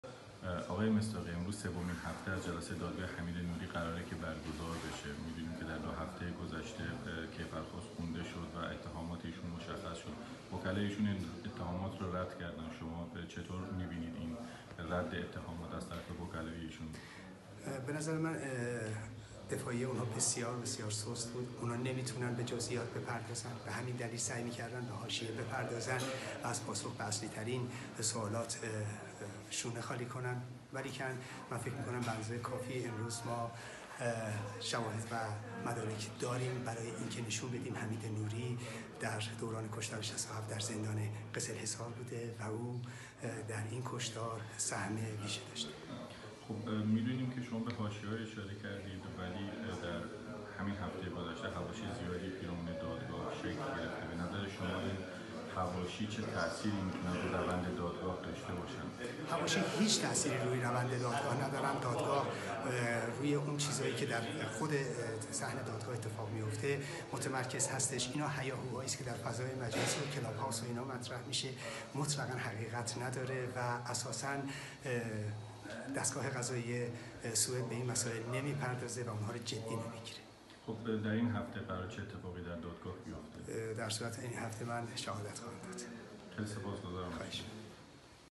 دادگاه حمید نوری: گفت‌وگو با ایرج مصداقی - هفته سوم ۲۳ آگوست/ اول شهریور
🔹ایرج مصداقی، زندانی سیاسی سابق در دهه ۶۰ و از شاکیان پرونده حمید نوری، در جریان سومین هفته دادگا،  توضیحاتی درباره روند دادگاه به زمانه می‌دهد.
🔹در هفته‌های گذشته حواشی بسیاری نگرانی‌ها را درباره تاثیر آنها بر روند دادگاه افزایش داد، مصداقی توضیح می‌دهد این حواشی بر روند دادگاه تاثیر نخواهد نگذاشت.